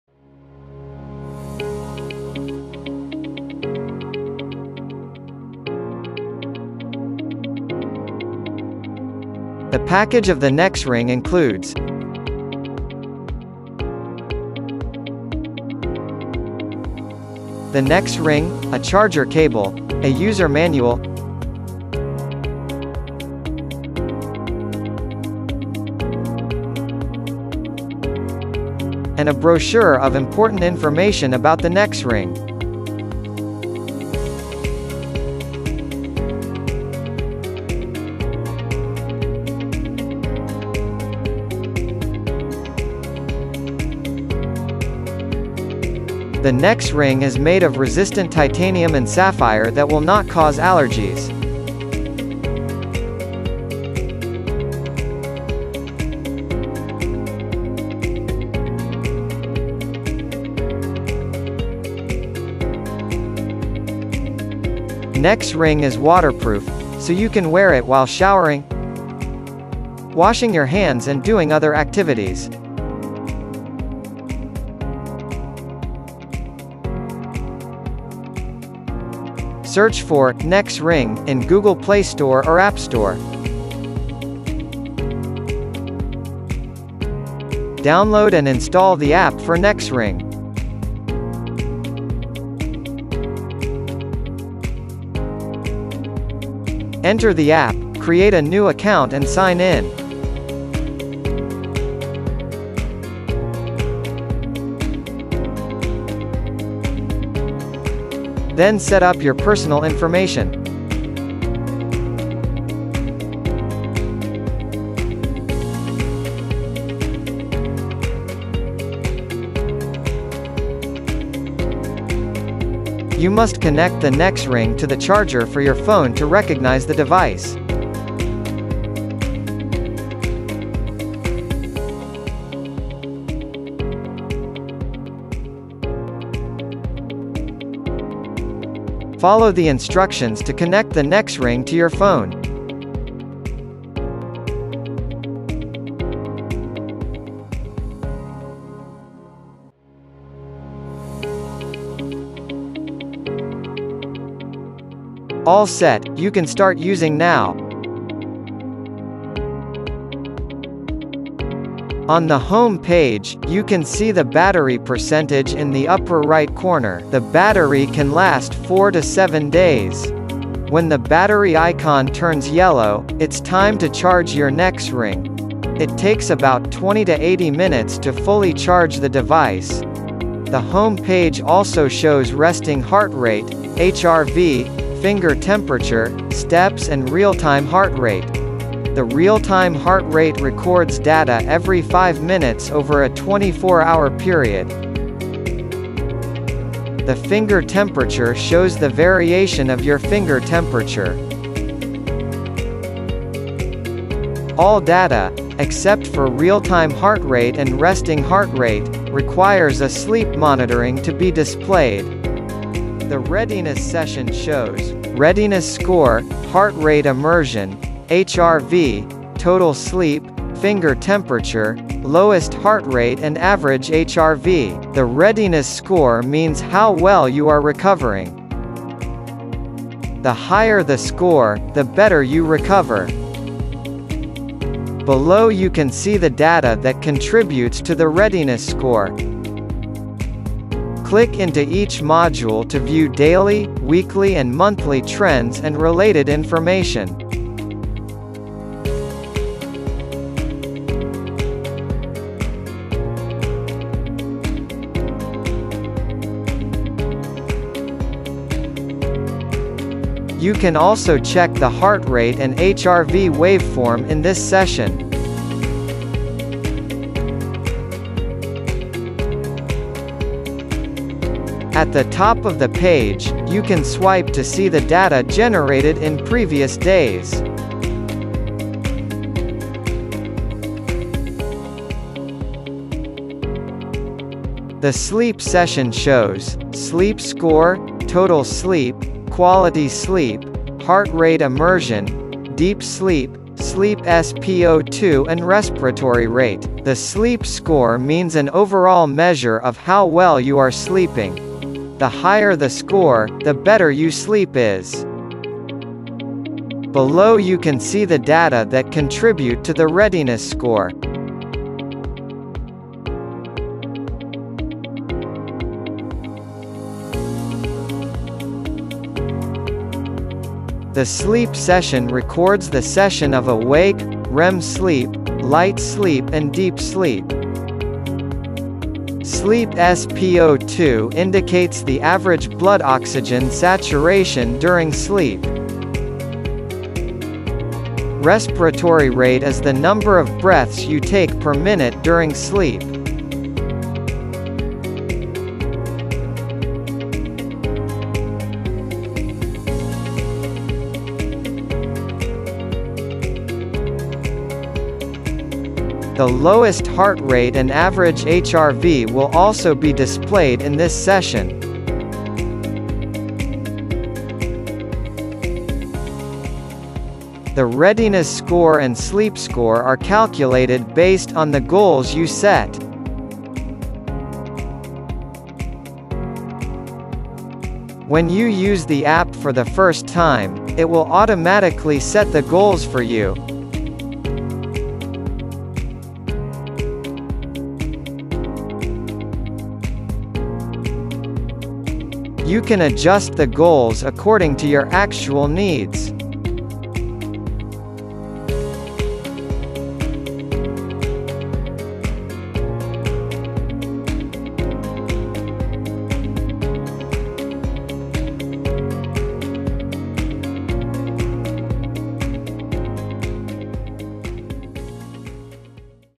Sleep and Wellness Tracking Smart Ring Unboxing | Linktop NexRing | Wearable Technology
Linktop NexRing could track SpO2, heart rate, HRV, finger temperature, step count, total sleep, quality sleep, and deep sleep. It presents readiness score, sleep score, and sleep session to help you understand your sleep & wellness condition and how ready you are for work and exercise.

Different from the Oura ring, NexRing accompanying app does not charge any membership fee. You feel free to check all the data and share the data with your doctor on the app anytime anywhere.

The smart sleep tracking ring is made of resistant titanium and decorated with sapphire. Currently we have size 7-13 for option.

The smart sleep and fitness tracker is a great gadget for those who have a sleep disorder, sleep apnea, and who care a lot about their general wellness and sleep health.

Video Chapters:
00:00 - Into, Overview
01:15 - Connect the device to app
02:39 -  Introduction of App home page
03:34 - Introduction of App readiness score page
04:47 - Introduction of App sleep score page
06:08 - Introduction of App setting page